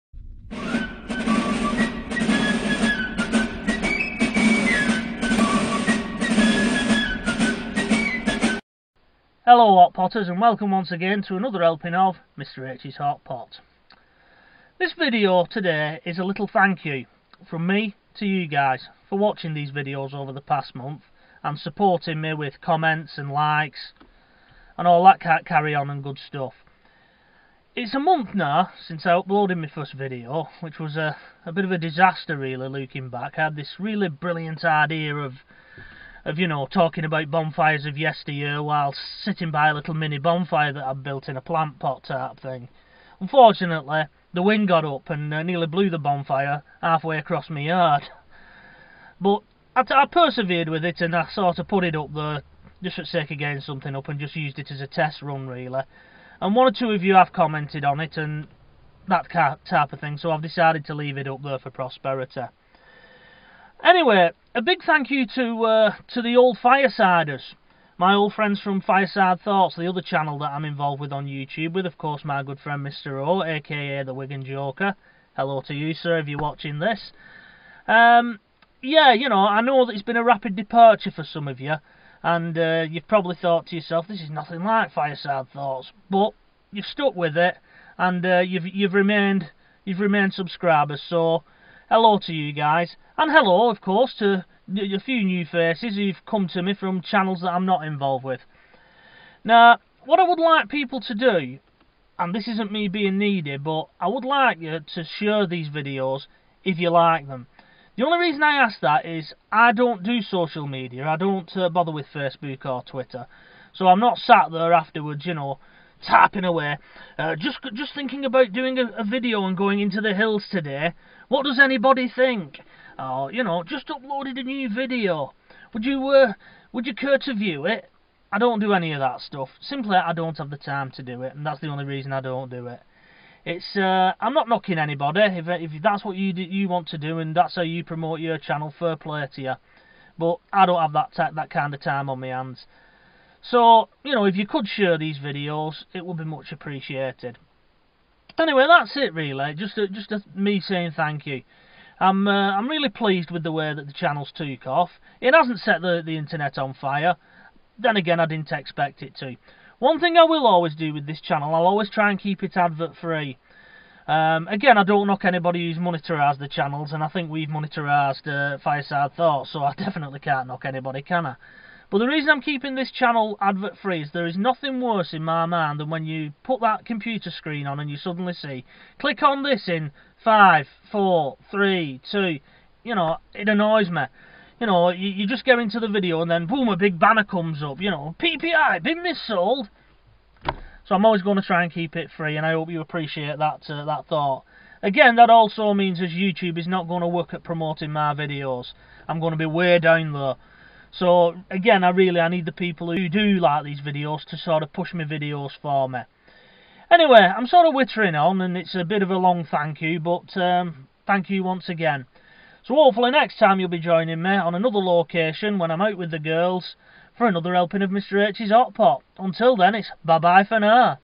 Thank you
Mr H marks the first month of his channel by making a special video to thank subscribers old and new who have supported him since starting 'Mr H's Hot Pot'.